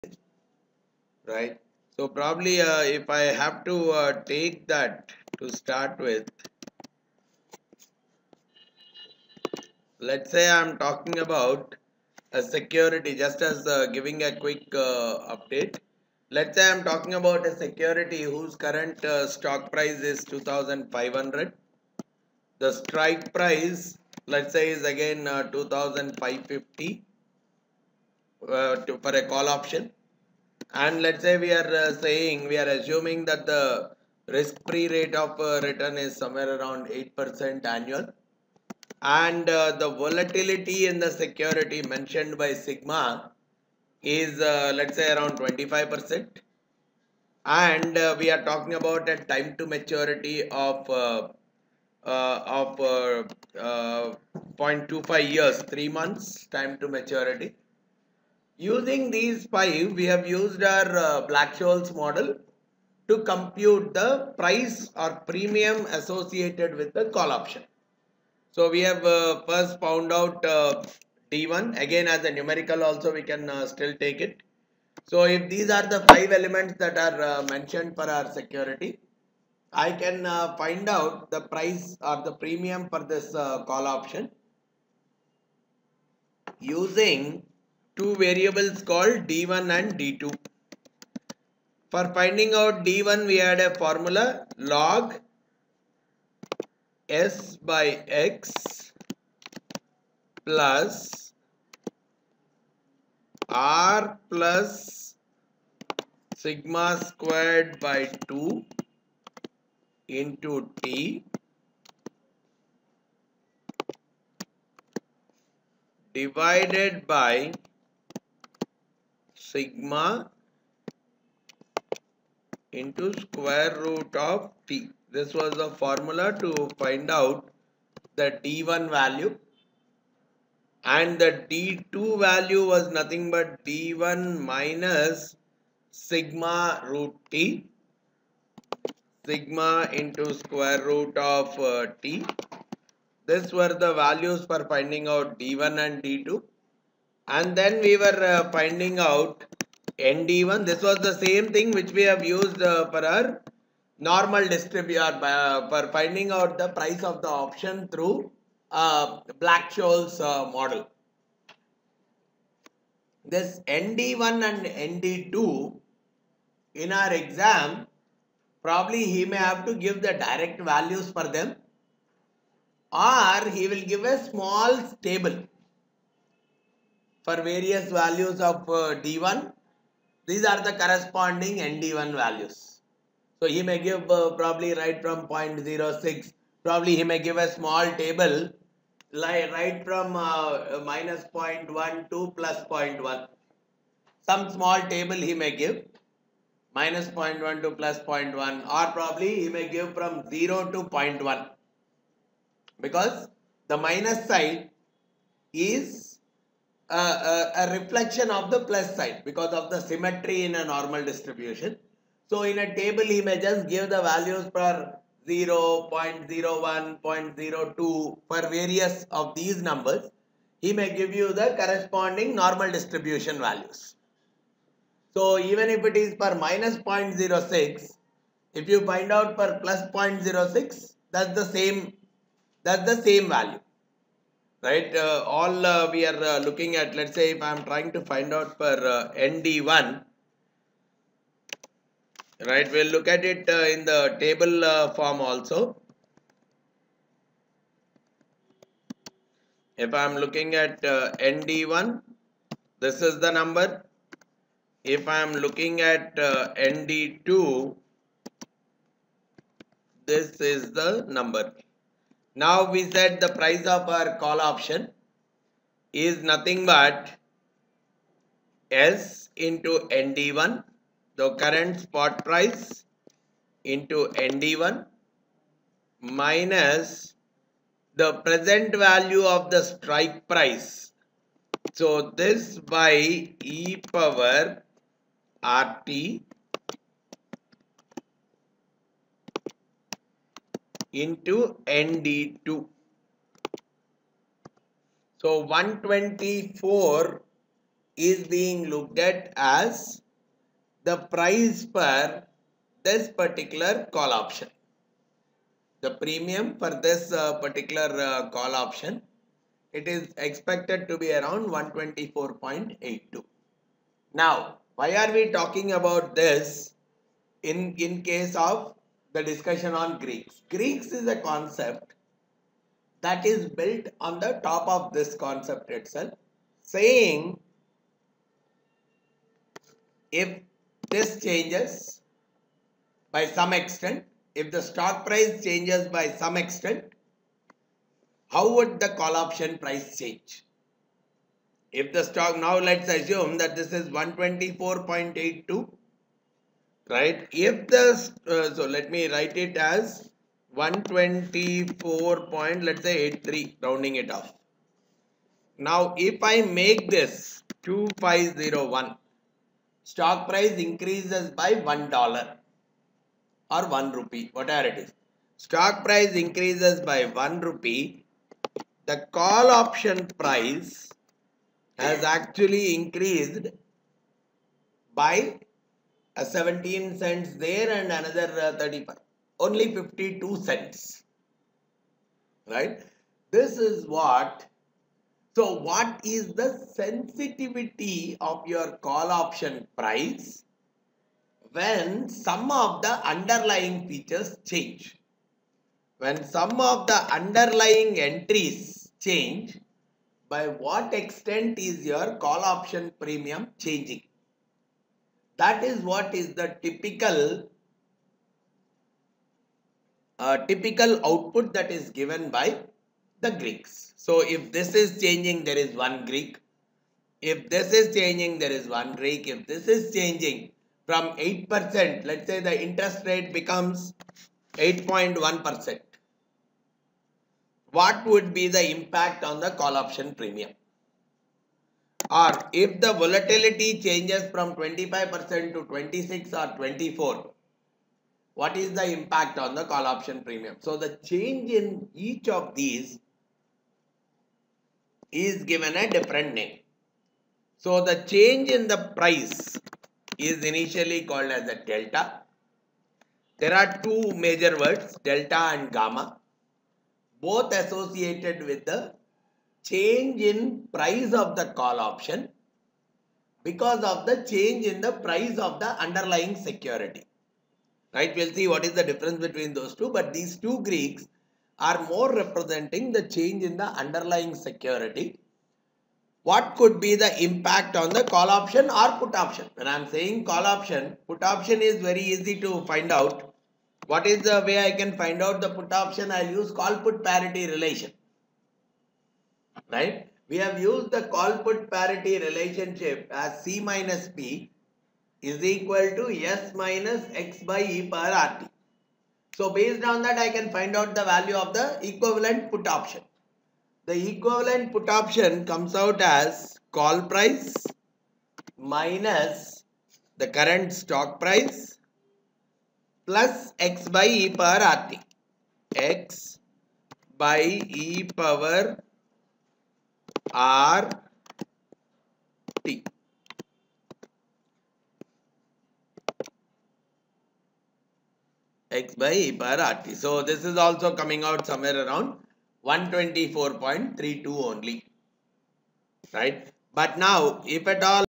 FRM part1 Greek Letters in Valuations and Risk Models
FRM Part 1 training at pacegurus by Vamsidhar Ambatipudi on Valuations and Risk Models.